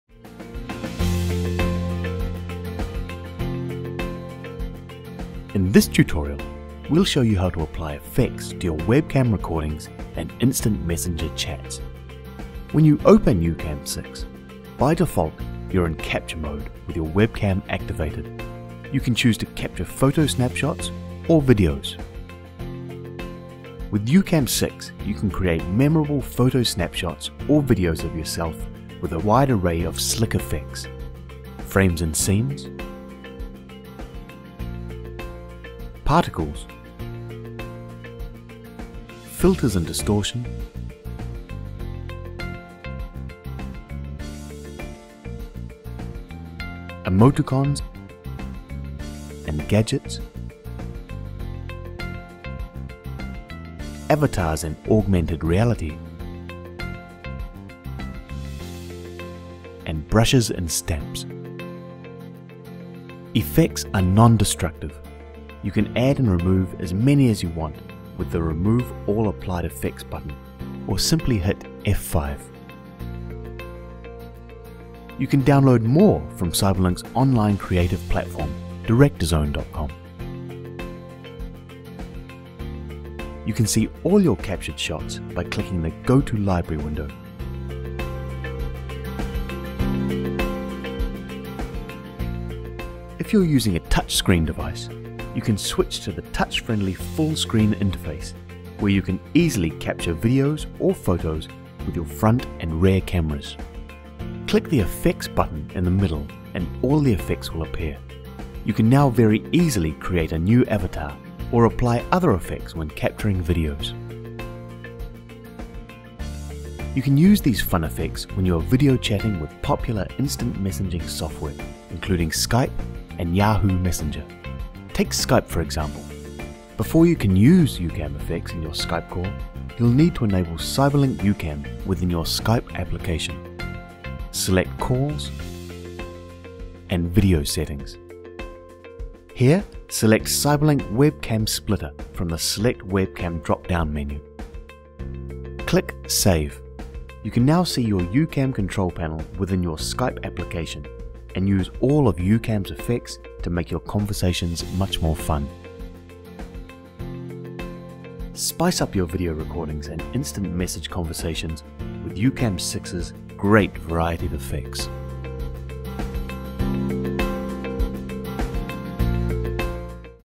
YouCam Tutorial - Apply effects for webcam recording & IM chatting | CyberLink
In this tutorial, we will show you how to apply effects to your Webcam Recordings and IM Chatting